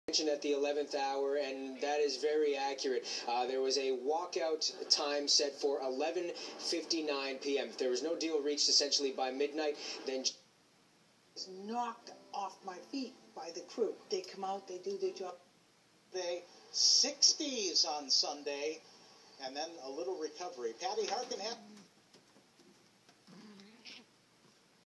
DTV Tropo - 11 CKWS Kingston, ON Received in Akron, OH
Distance is 345 miles, received via tropospheric ducting on 9/20/16